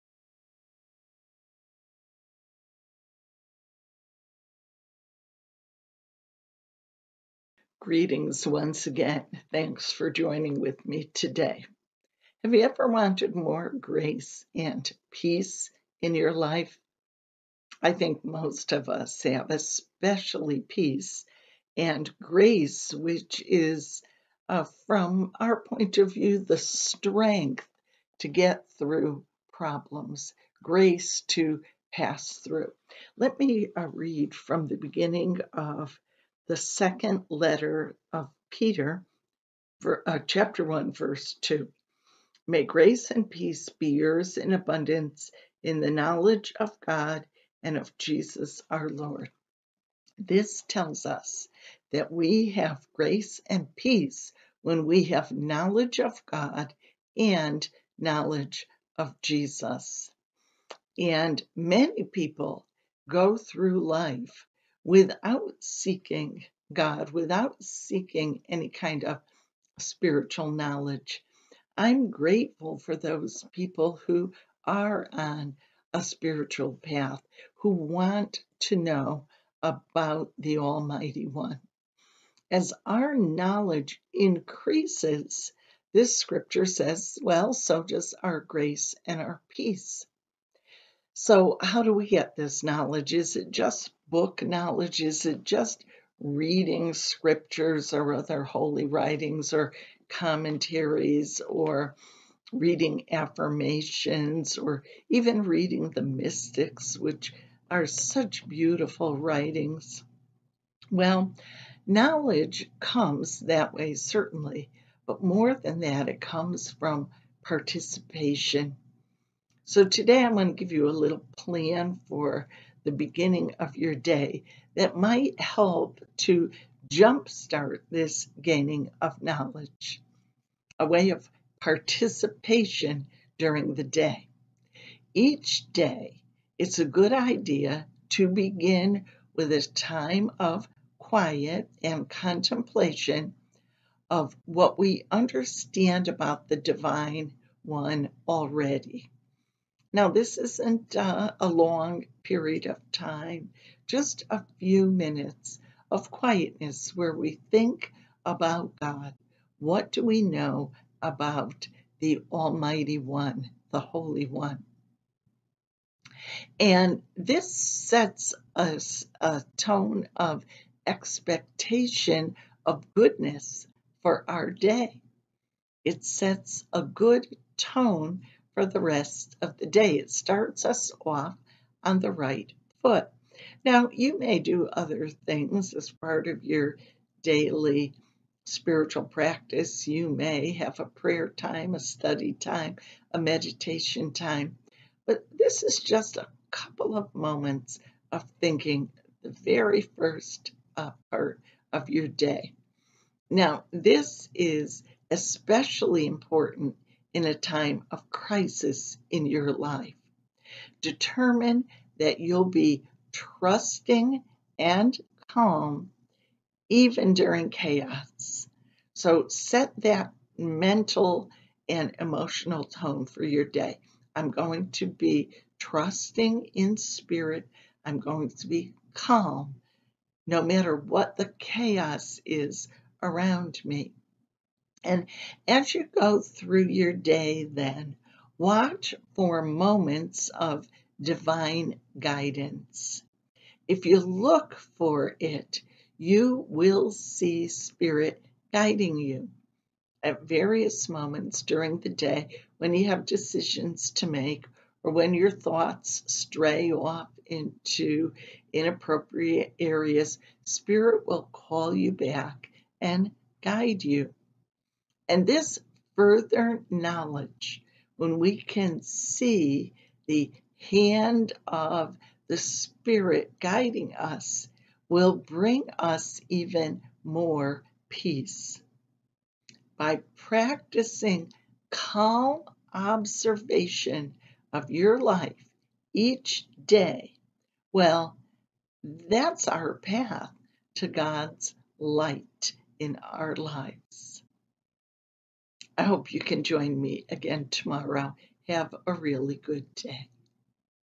June 11 Daily Devotional The Way of Peace
As our knowledge of God and Jesus increases so does our grace and peace according to 2 Peter 1:2.